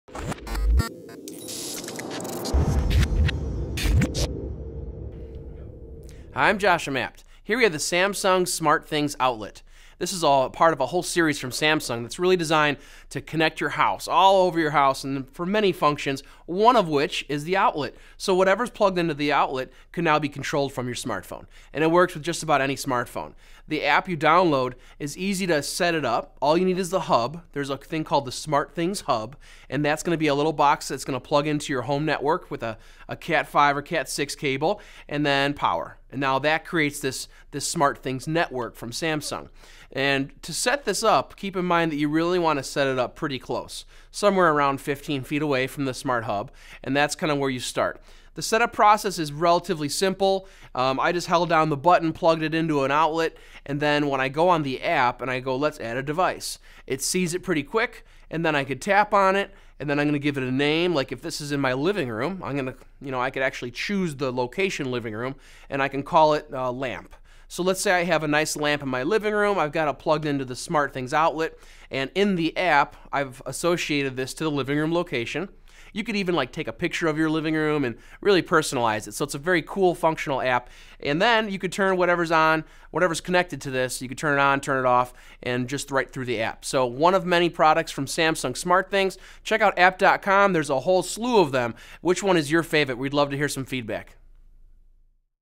Samsung SmartThings Power Outlet F-CEN-APP-1- Overview
Samsung SmartThings Power Outlet - F-CEN-APP-1

The Samsung SmartThings Outlet turns any regular three-prong outlet into a smart controllable power outlet. By plugging in this portable device you’ll be able to control and automate lamps electronics and small appliances from another room or another country directly from the free SmartThings app.
Key Features:
• Control lights Electronics and Small Appliances from Your Smartphone
• Trigger Lights to Automatically Turn On and Off
• Power Things Like Coffee Machines and Portable Heaters with Your Smartphone
• Save Energy by Restricting Power to Electronics or Appliances
Features:
• Control From Your Smart Phone
• Trigger Automatic Actions
Trigger lights to automatically turn on when you come home and turn off when you leave.
• Power Appliances From Your Smart Phone
Power things like coffee machines Crock-Pots A/C units portable heaters and fans using your smartphone.
• Extend The Range of Your Devices
Acts as a ZigBee repeater extending the range of your ZigBee devices when plugged into a wall outlet.